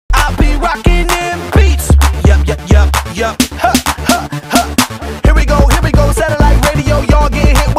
Cotton Tail Bunny Jumps for Joy! 🐰
When Bucky the bunny gets excited, he lets us know by doing a few "binky" bunny hops.